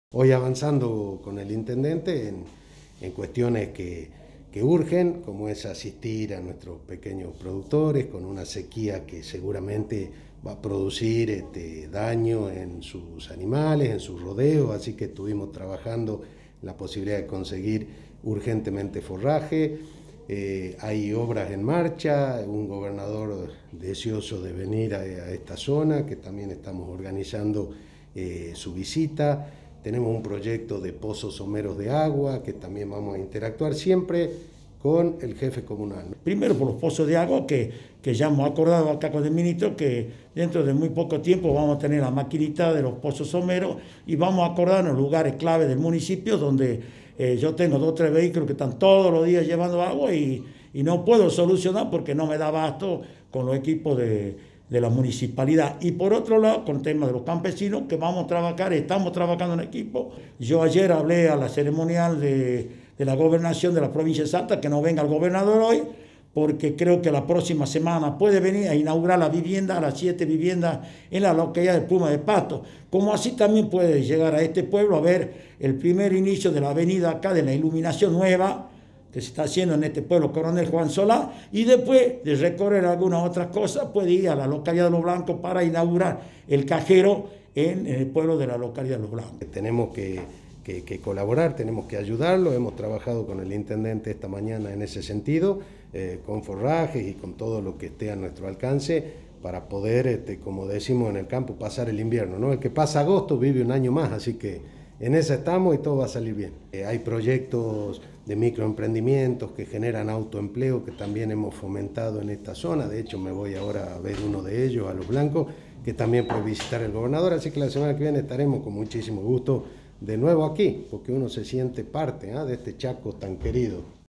Trabajan en soluciones para mitigar el impacto de la sequía en Rivadavia Banda Norte
Nota al ministro de Producción, Martín de los Ríos y al intendente de Coronel Juan Solá, Atta Gerala.